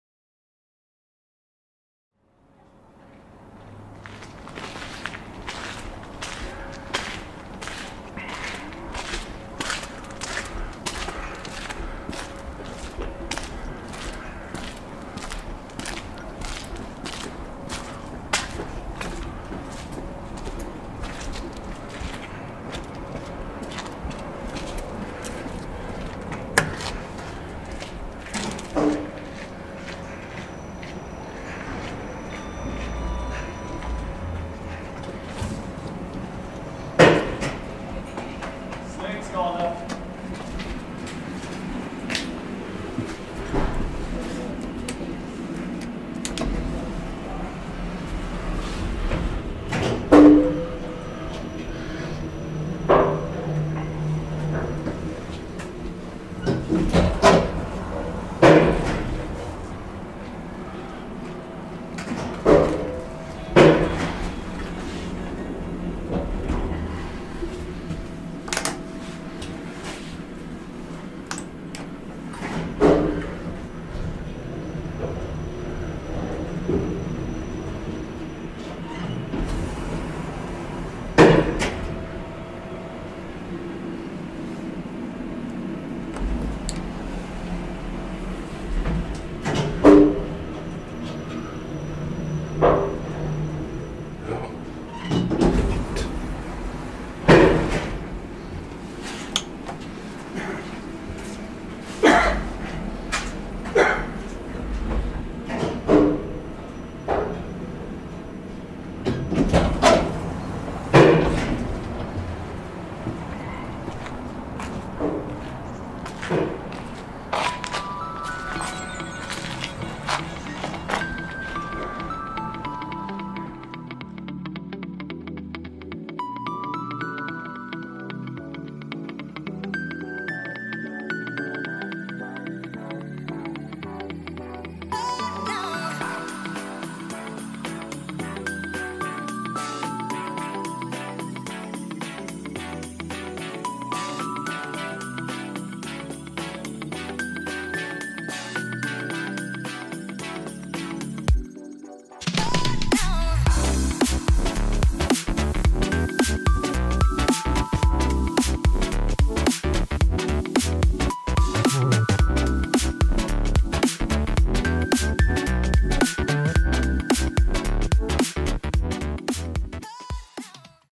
2003 KONE MonoSpace MRL-Traction Outdoor Elevator @ Primusgatan-Luxgatan, Stockholm, Sweden!
ELEVATOR INFORMATION!

Manufacturer/Tillverkare: KONE
Model/Modell: MonoSpace Delta
Installation year/Byggår: 2003
Capacity/Maxvikt: 1050Kg - 14 Persons
Floors/Antal våningar: 5 = (1 2) E 3 4

Location/Adress: It connects between Primusgatan & Patentgatan with Luxgatan - 112 67 Stockholm (Lilla Essingen) SWEDEN!
Date of recording/Inspelningsdatum: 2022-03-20 with Panasonic HC-V7FX990 & RØDE Stereo Videomic Pro!